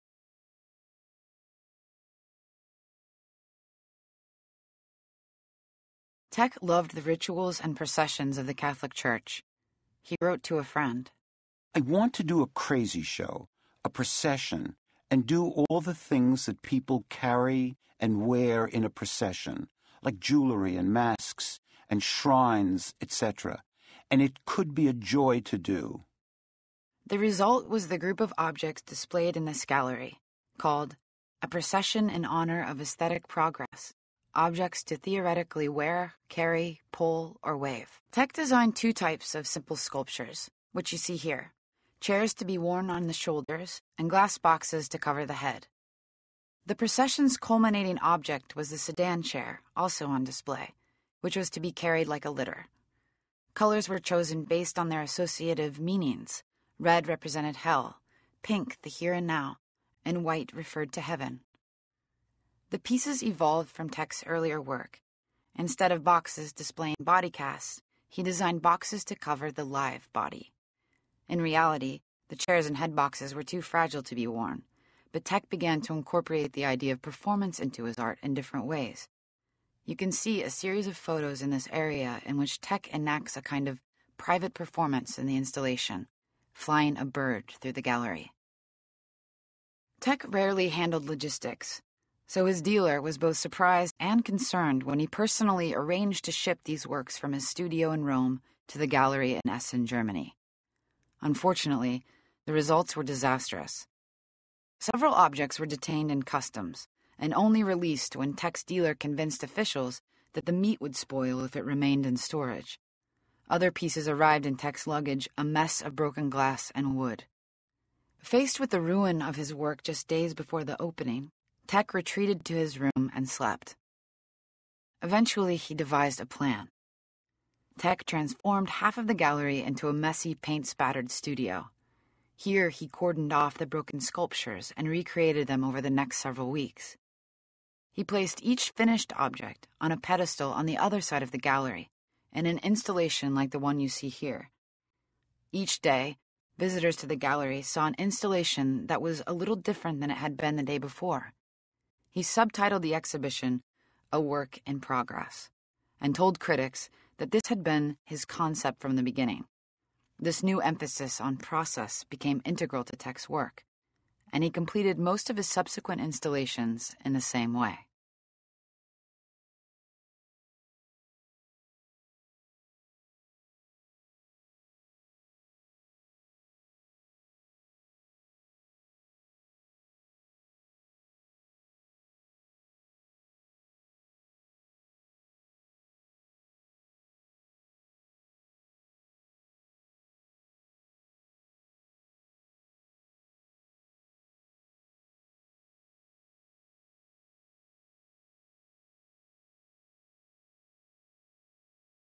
Paul Thek: Procession
In this video, the artist Paul Thek's "Procession in Honor of Aesthetic Progress" (1968), on view in the exhibition "Paul Thek: Diver, A Retrospective" is discussed.